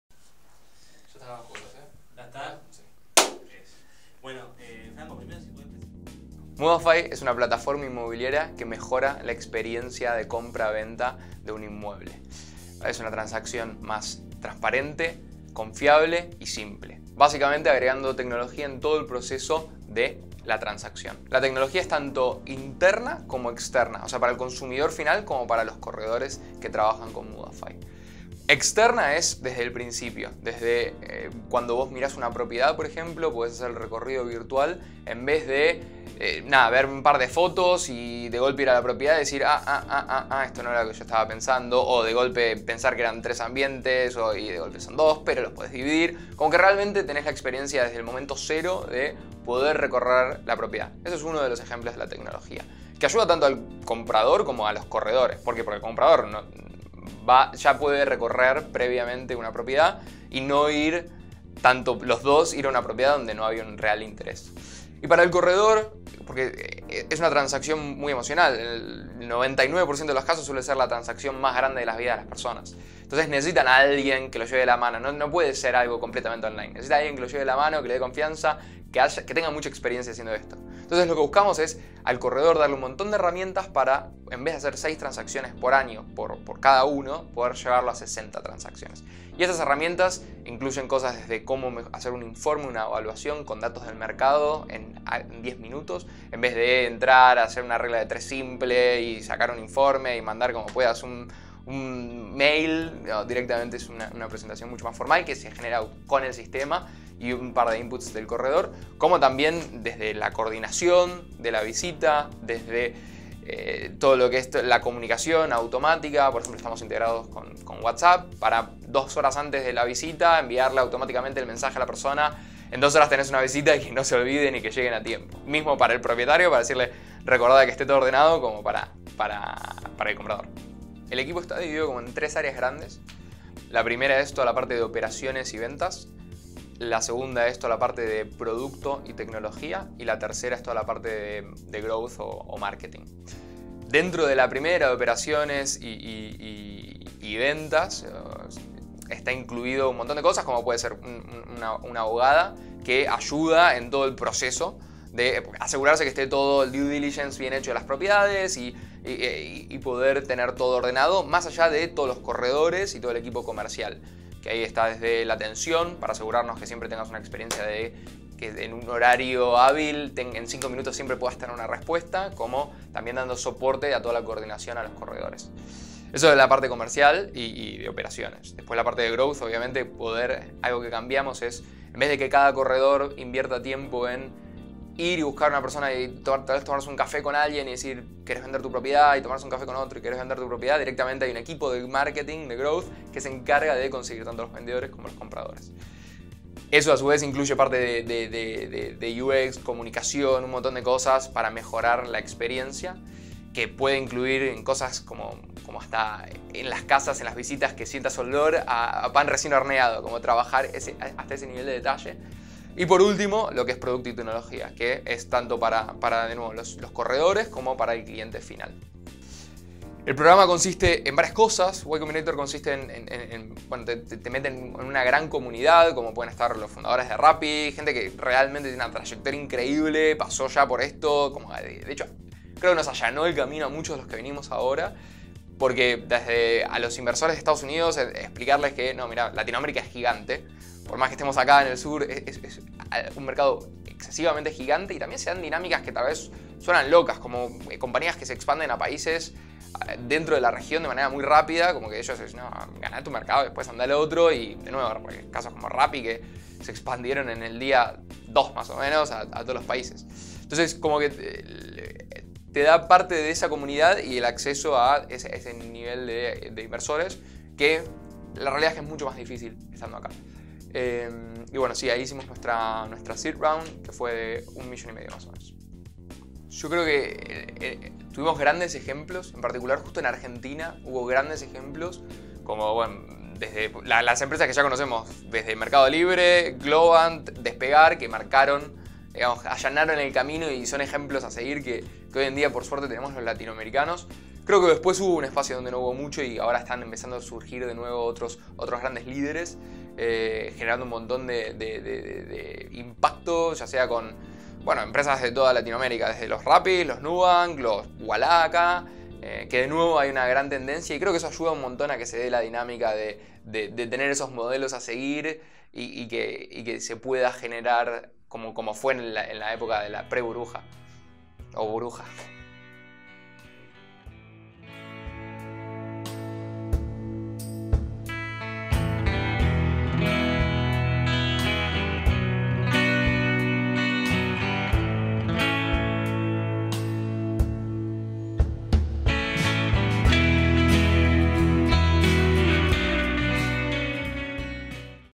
Franco Forte: Mudafy
Mudafy es la plataforma que facilita la compra y venta de propiedades. Conocé a Franco Forte, uno de los dos fundadores de este gran emprendimiento que se potenció en "Y Combinator" una de las grandes aceleradoras de Silicon Valley.